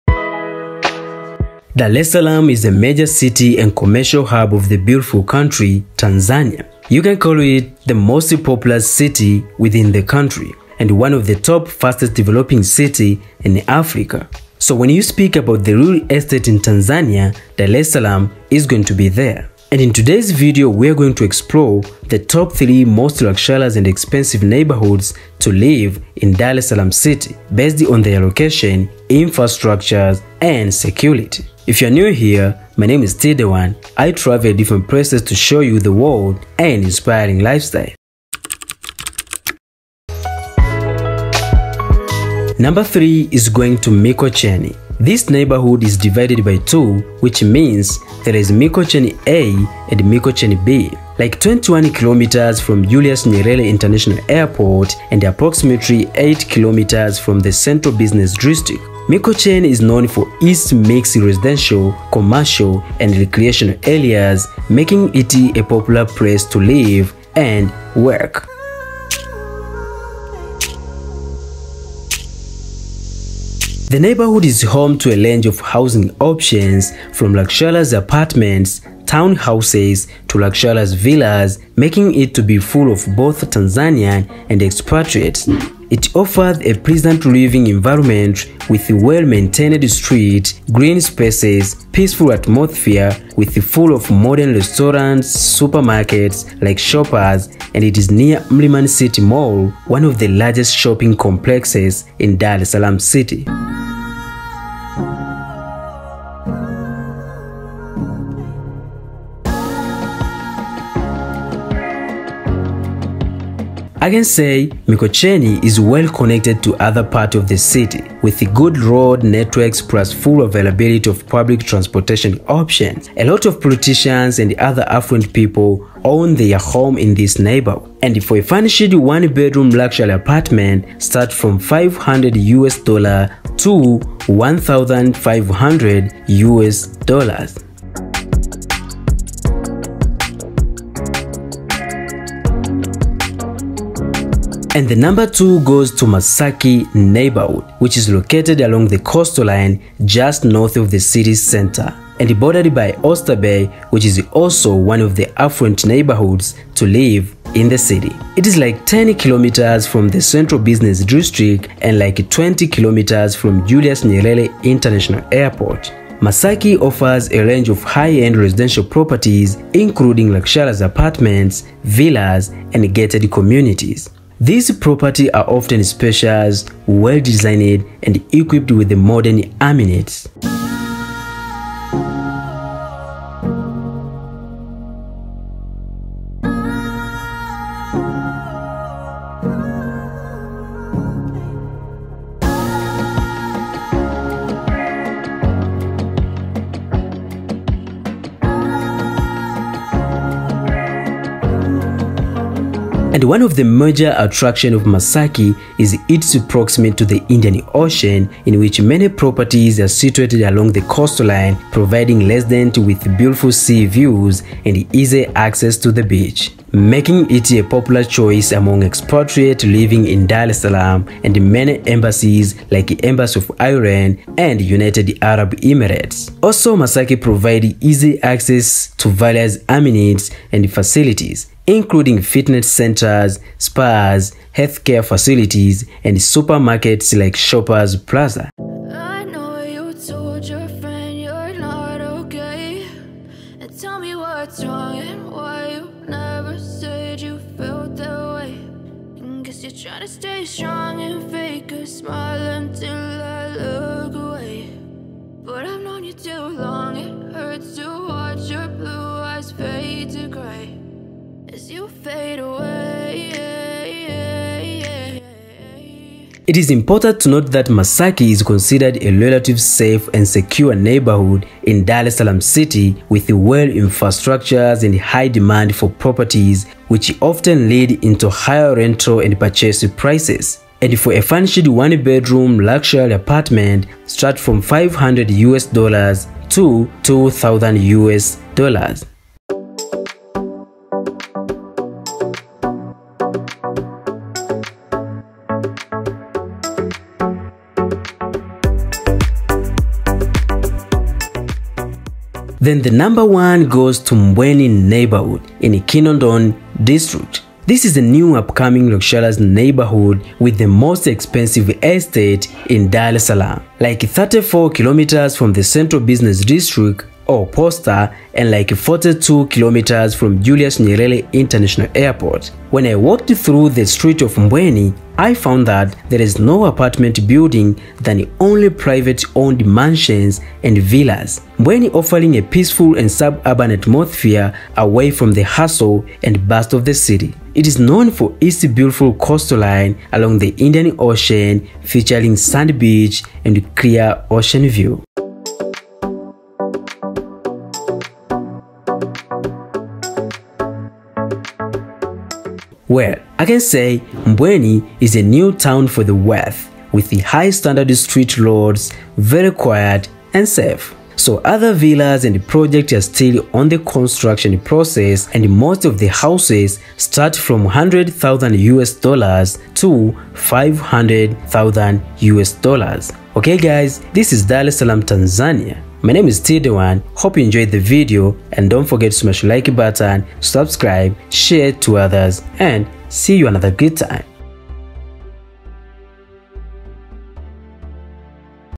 Top 3 Richest Sides of Tanzania 🇹🇿 Where The Riches Hide in Dar es Salaam
In this Video I present to You the Most Luxurious & Expensive Neighborhoods to Live in Dar es Salaam City Tanzania 🇹🇿

Some of Mbweni Footages Credit to @explorewithbertin